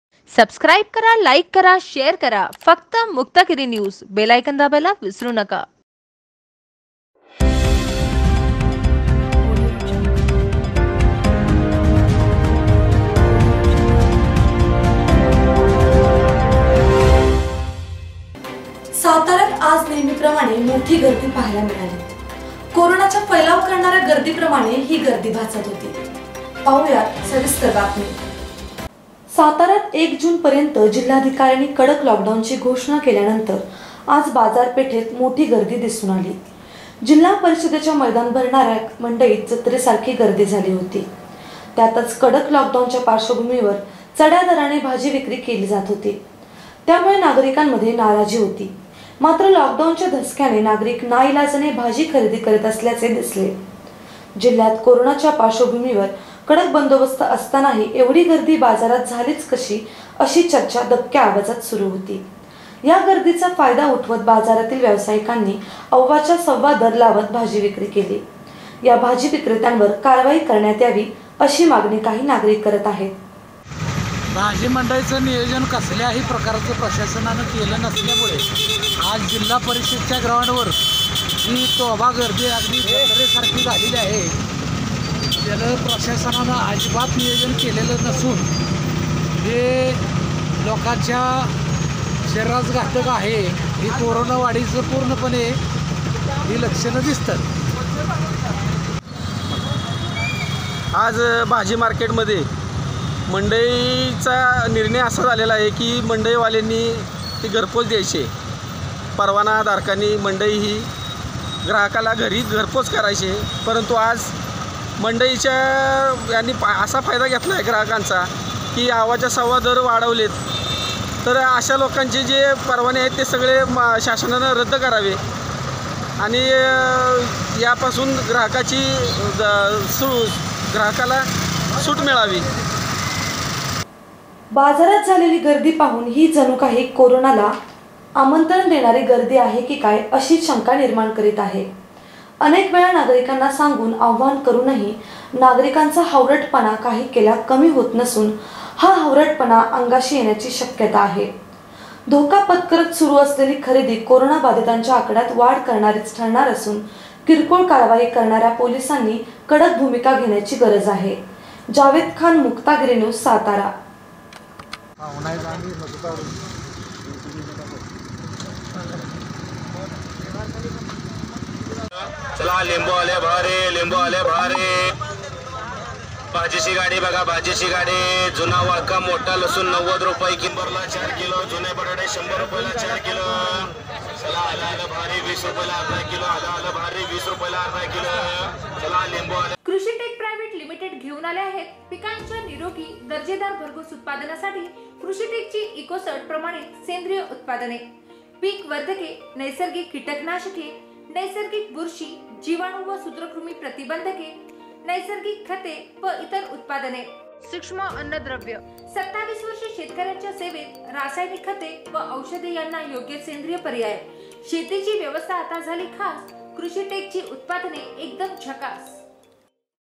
साताऱ्यात भाजी मंडईत नागरिकांची तुडुंब गर्दी ; भाजी विक्रेत्यांनी चौपट भावात विकली भाजी ...
साताऱ्यात भाजी मंडईत नागरिकांची तुडुंब गर्दी ; भाजी विक्रेत्यांनी चौपट भावात विकली भाजी  ... रोजच्या ताज्या बातम्या पाहण्यासाठी आजच लाईक करा शेअर करा आणि सबस्क्राईब करा फक्त मुक्तागिरी न्यूज ... बेल आयकॉनवर क्लिक करायला विसरु नका ... जावेद खान सातारा ...